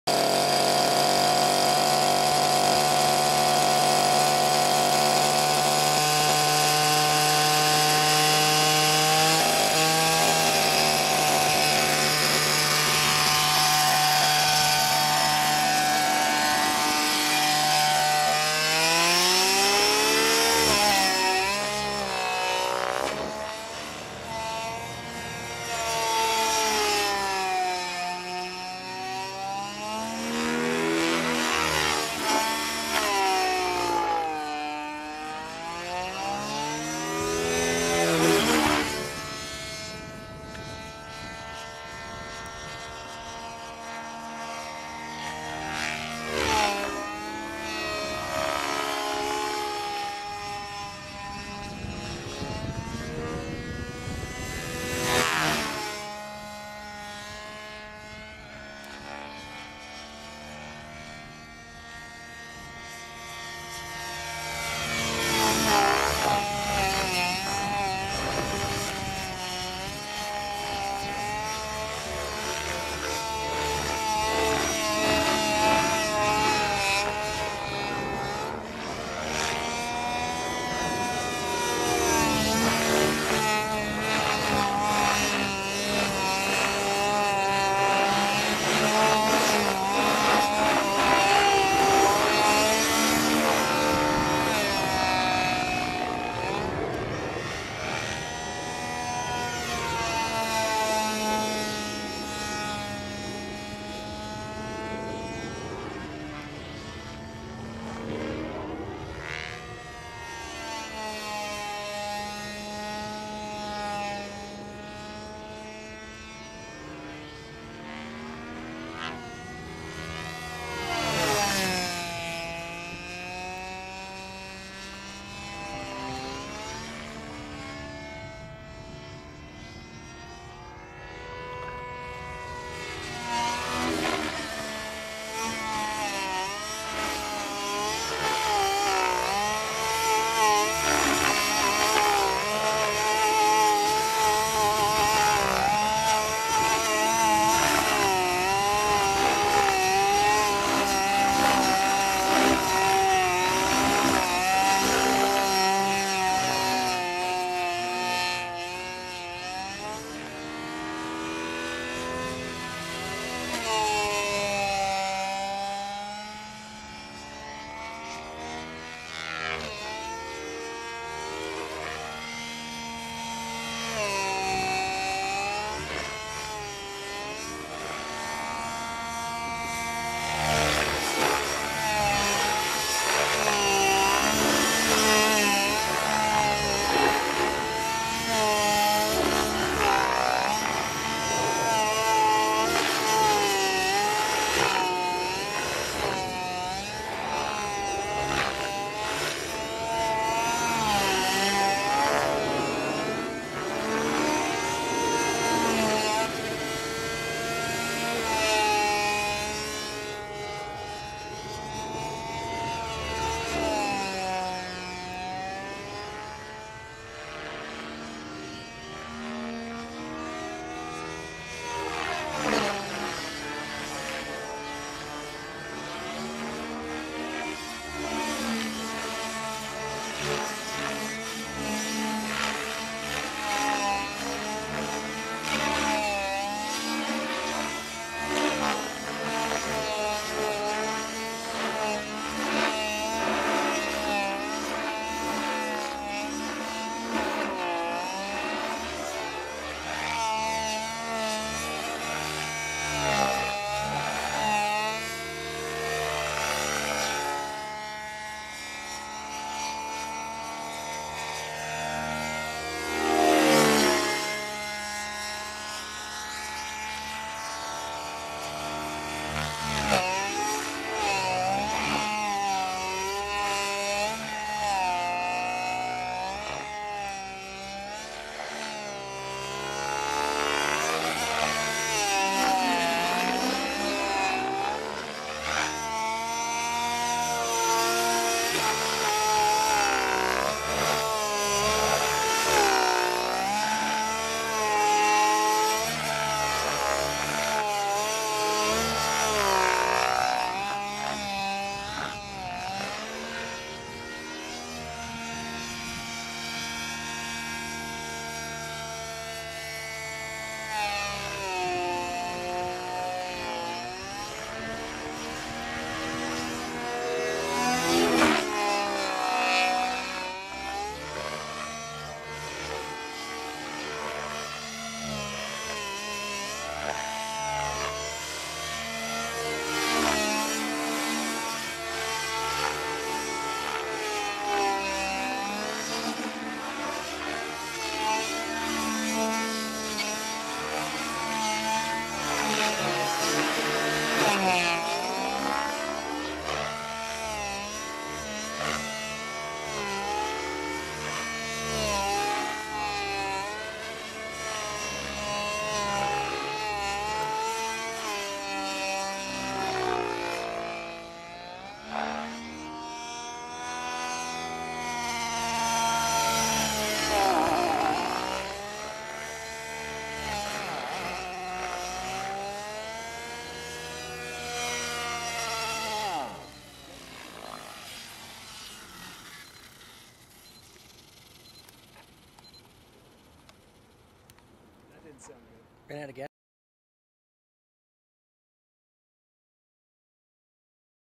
Radikal G30 v.2 w/HWC 3D Extreme Pro 29
Century Radikal G30 v.2 running a HWC 3D Extreme Pro 29cc Gasoline Engine

Rotortech 690mm Main Blades
Rotortech 105mm Tail Blades
Savox HV Servos
Thunder Power RX Battery
V-Bar
6.0 Gear Ratio

Pilot: Garett Oku